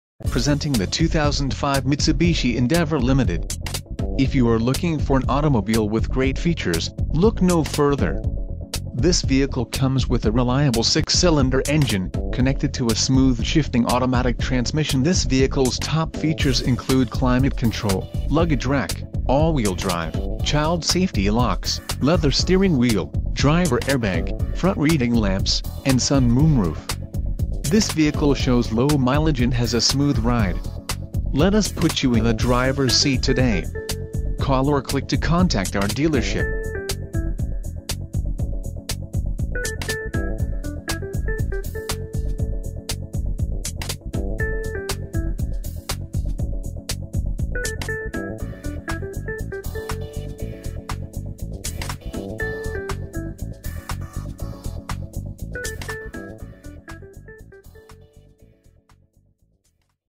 2005 Mitsubishi Endeavor - Lone Star Chrysler Dodge Jeep Ra
For Sale in: Cleburne, TX

 Presenting the 2005 Mitsubishi Endeavor Limited. If you are looking for an automobile with great features, look no further. This vehicle comes with a reliable 6 cylinder engine, connected to a smooth shifting automatic transmission.. This vehicles top features include Climate Control, Luggage Rack, All Wheel Drive, Child Safety Locks, Leather Steering Wheel, Driver Air Bag, Front Reading Lamps, and Sun/Moon Roof. This vehicle shows low mileage and has a smooth ride. Let us put you in the drivers seat today. Call or click to contact our dealership..